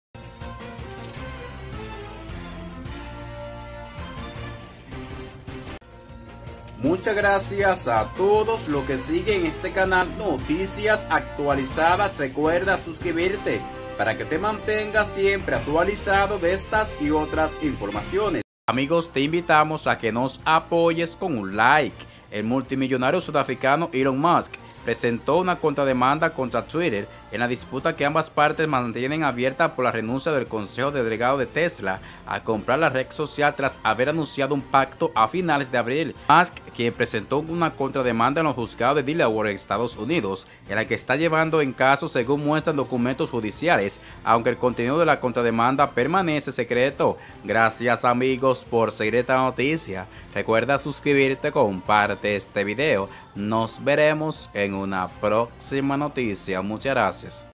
ELON MUSK CONTRADEMANDA A TWITTER EN SU DISPUTA POR LA COMPRA DE LA RED SOCIAL
El multimillonario sudafricano Elon Musk presentó este viernes una contrademanda contra Twitter en la disputa que ambas partes mantienen abierta por la renuncia del consejero delegado de Tesla a comprar la red social tras haber anunciado un pacto a finales de abril.

Musk presentó una contrademanda en los juzgados de Delaware (EE.UU.) en que se está llevando en caso, según muestran documentos judiciales, aunque el contenido de la contrademanda permanece secreto.

El juicio entre Twitter y Musk para resolver la disputa por la compra de la red social comenzará el 17 de octubre a menos que ambas parten lleguen a un acuerdo sobre otra fecha, según documentos del caso hechos públicos también este viernes.

La jueza que lleva el caso en un tribunal de Delaware ordenó el jueves a última hora que el juicio se celebre a partir de ese día, tras acceder previamente a la petición de Twitter de un proceso por la vía rápida frente al reclamo de Musk de posponerlo a febrero del año que viene.